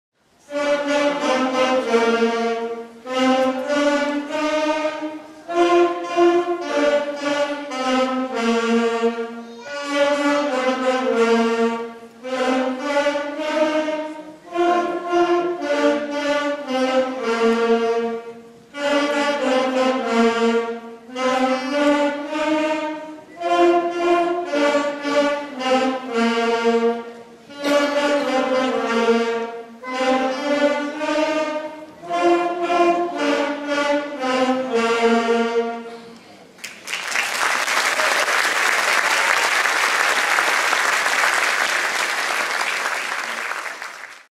Saxophone and French Horn Sections
The Fifth Grade All-City Band Concert May 29, 2013